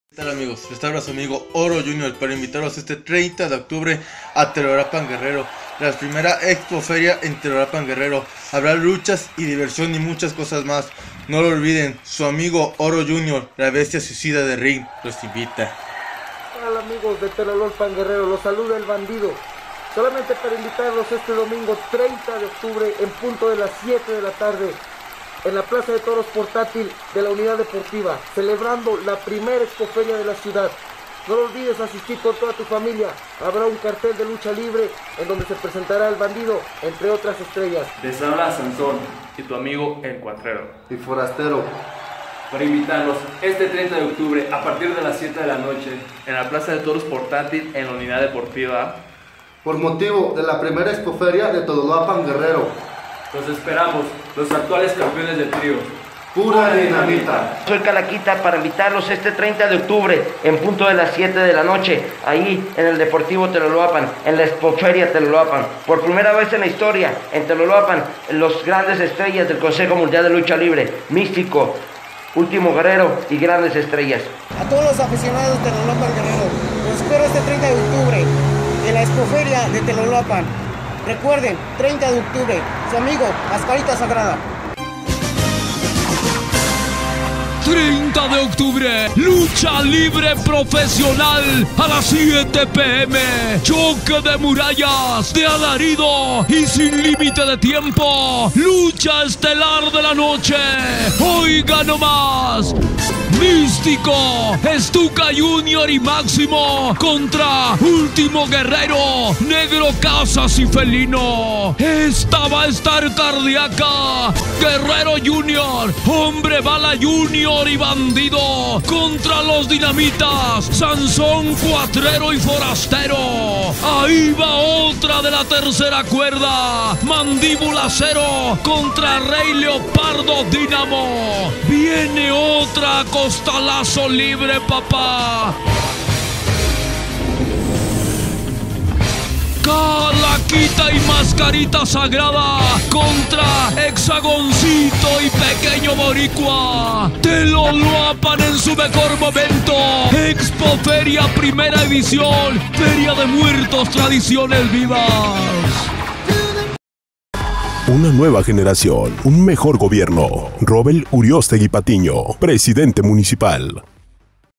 Lucha Libre Mexicana en la Expo Feria Teloloapan 2016. 30 Octubre 7 P.M.
Preventa $50 pesos. Estrellas del Consejo Mundial de Lucha Libre

Lucha Estelar:
Místico, Máximo y Stuka Jr. VS  Último Guerrero, Negro Casas y El Felino.
Lucha.
Oro Jr., Bandido y Hombre bala Jr. VS  Los Dinamita (Sansón, Cuatrero y Forastero).
Lucha Estelar de Minis
Mascarita Sagrada y Kalakita VS Pequeño Boricua y Hexagoncito.
Lucha
Mandíbula Zero vs Rey Leopardo y Dinamo.

Te invita tu amigo, Robell Uriostegui Patiño, Presidente Municipal de Teloloapan, Guerrero, México